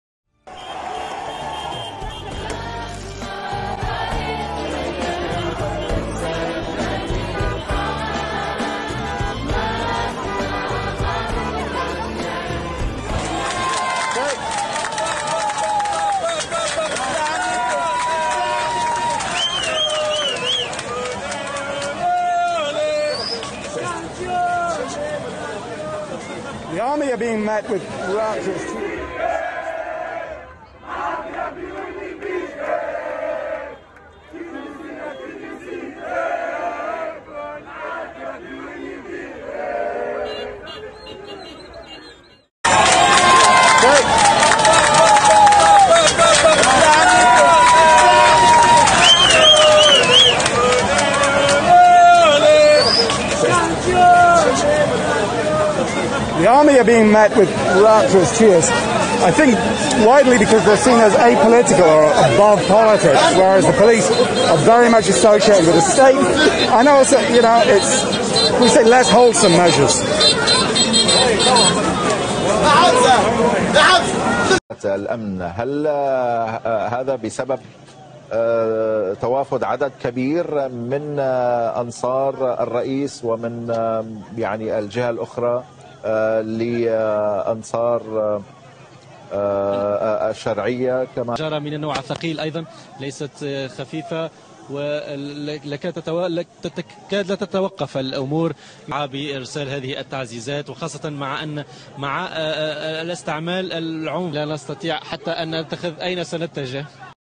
У Тунісі - заворушення. Що відомо
У Тунісі демонстранти зібралися під стінами будівлі Асамблеї народних представників.  Напередодні офіси правлячої партії «Ан-Нахда» брали штурмом.  За повідомленнями ЗМІ, сьогодні вони теж піддавалися атакам.

Телеканал Al Jazeera заявив, що поліція увірвалася в офіс і конфіскувала техніку.  Причини не повідомляються.  Сили безпеки заявили, що діють згідно з інструкціями, і попросили журналістів вийти.

 Раніше президент Тунісу Каїса Саїд відправив у відставку прем'єр-міністра і заморозив роботу парламенту.  Депутатам також заборонили покидати країну.  Спікер Аль-Ганнуші звинуватив президента в «держперевороті».

Протести в країні почалися через невдоволення економічною кризою і тим, як уряд дає раду з пандемією коронавіруса